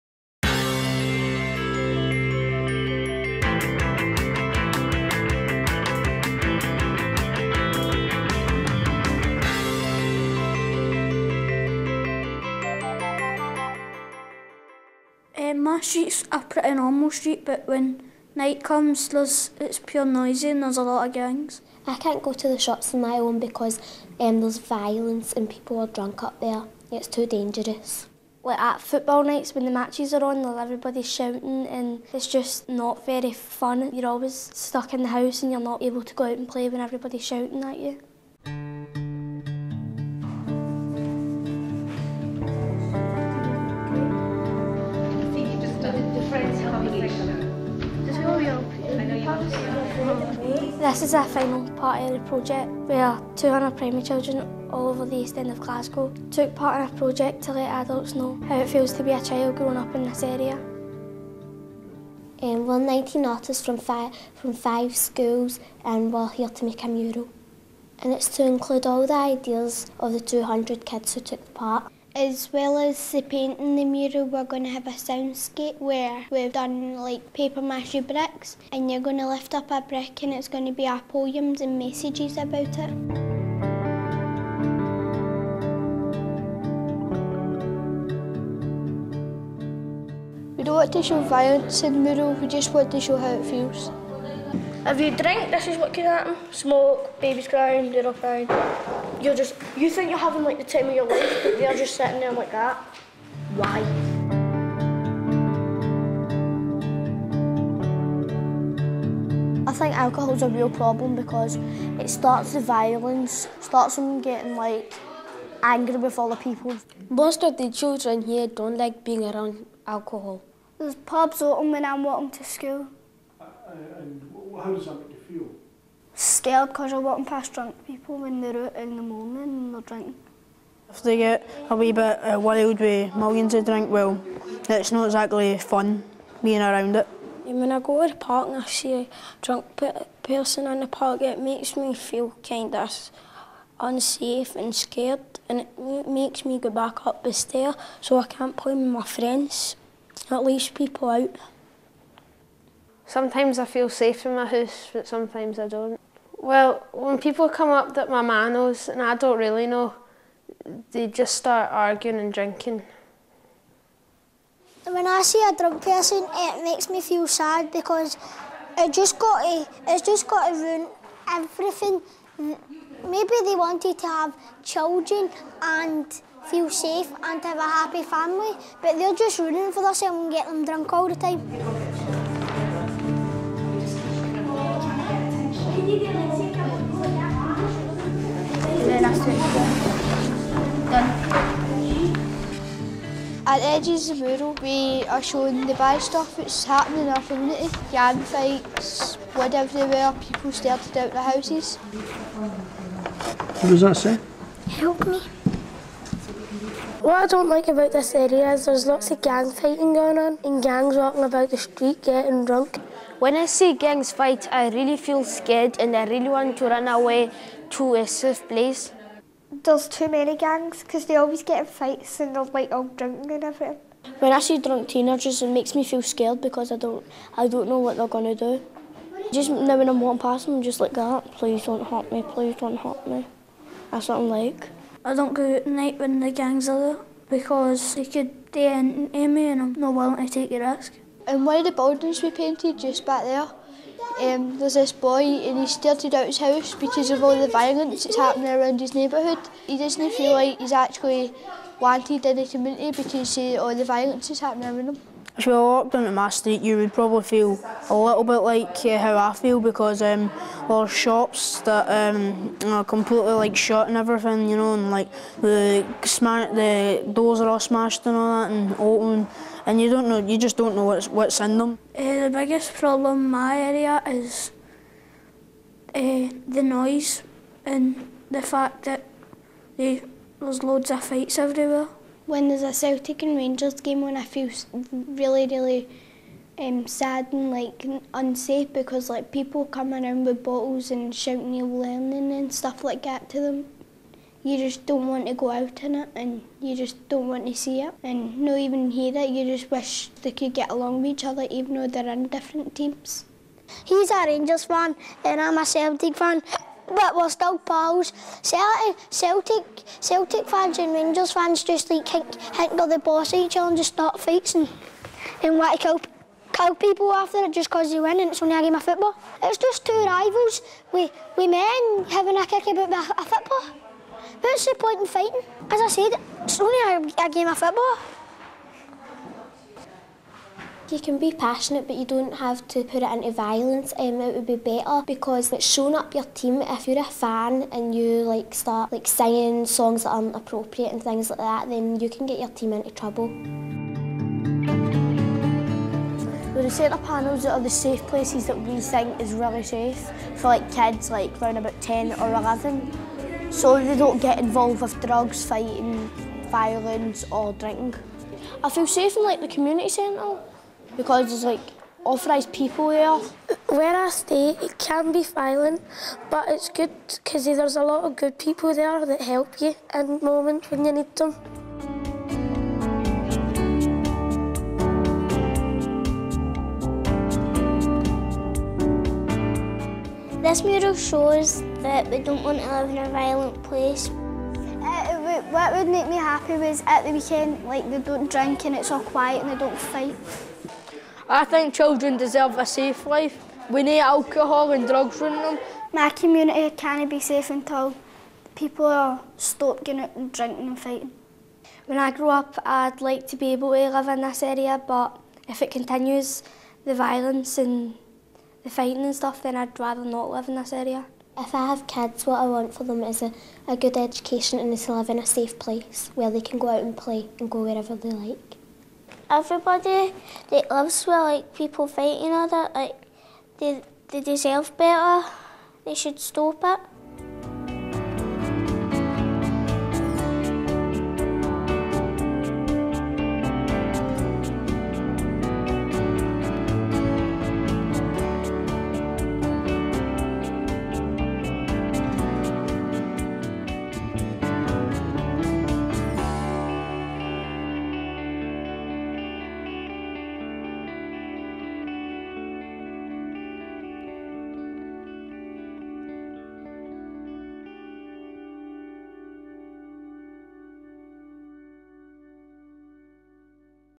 StreetsAhead Glasgow (2011)
Children's Parliament worked with children in the East End of Glasgow to bring their views on what their communities need to be safe, healthy and happy.

This purpose of the StreetsAhead Glasgow Mural Painting week film is to give the children the opportunity to talk about life in their community.

"An absolutely outstanding film with a really powerful message. It ticks all the boxes -- alcohol, violence, sectarianism, football."
John Carnochan, Co-founder of the Violence Reduction Unit, Scotland.